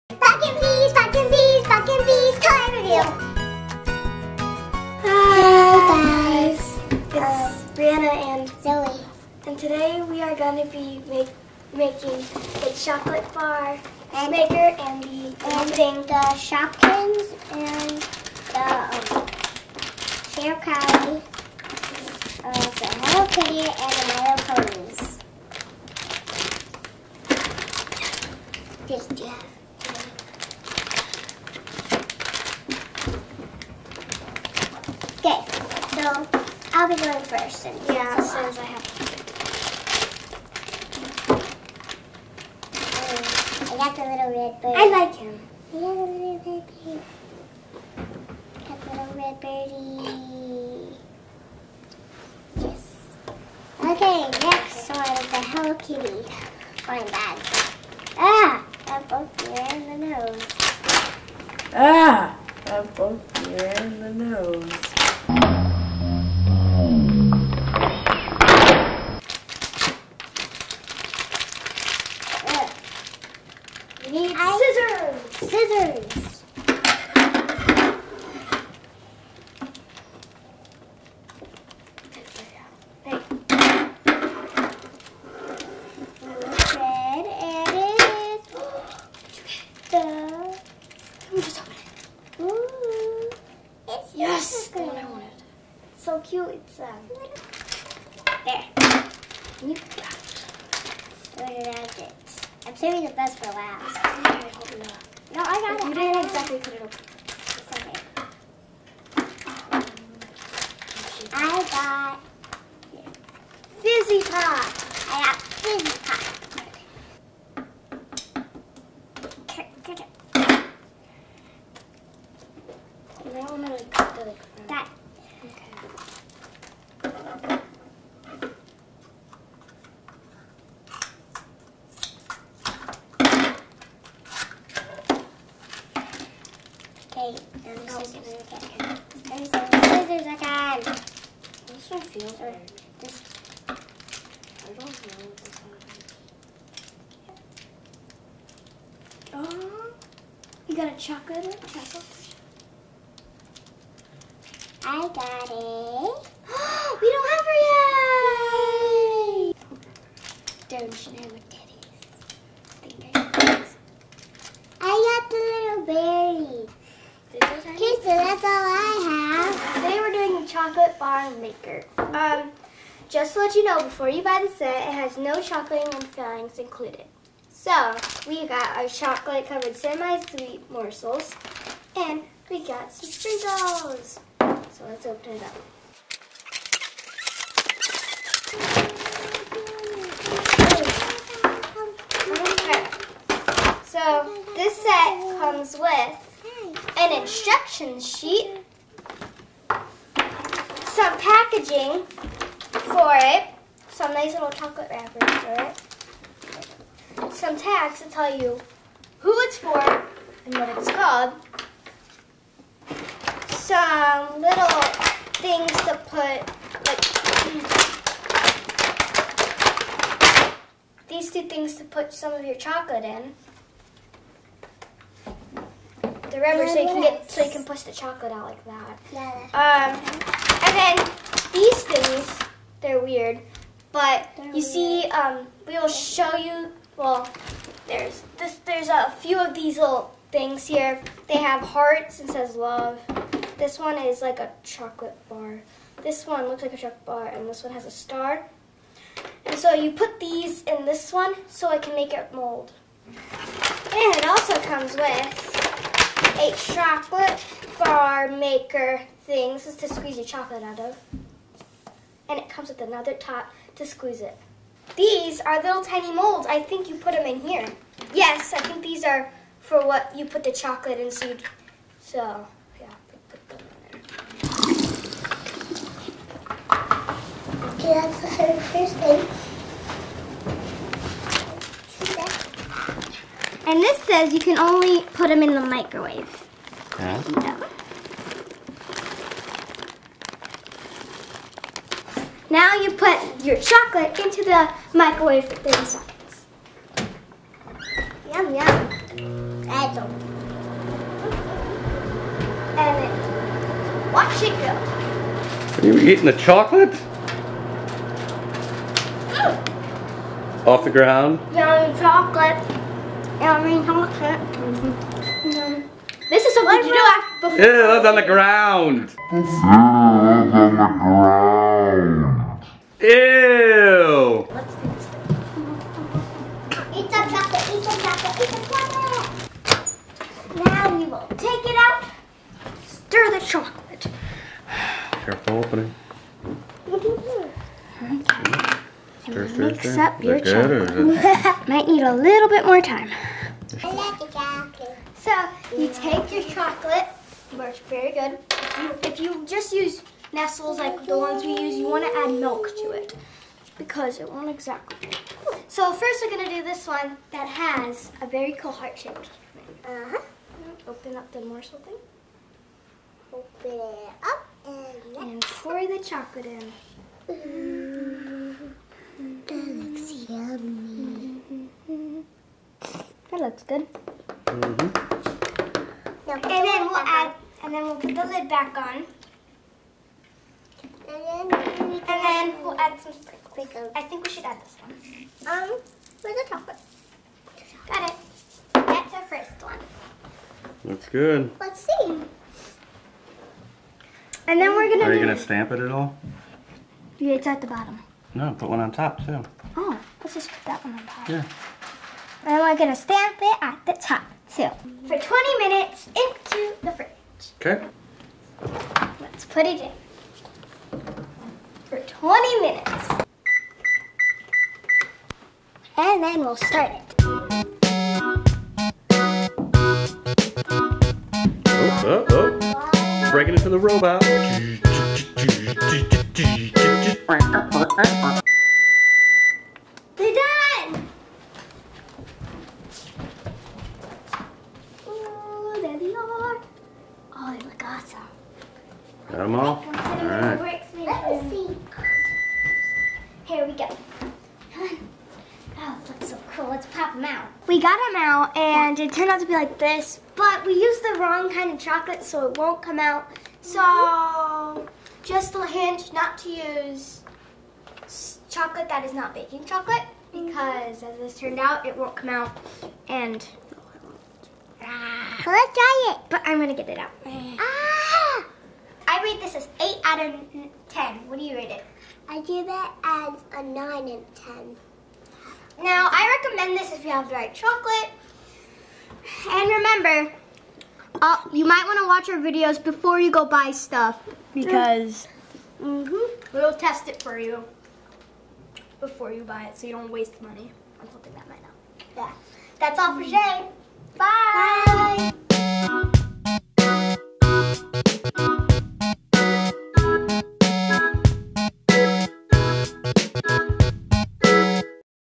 OUR FIRST SHOW! Chocolate Bar Candy Maker Review - Jan 30 2016
Today we review the Chocolate Bar Candy Maker, plus, we crack open some blind bags! Enjoy the show, and give us your feedback below!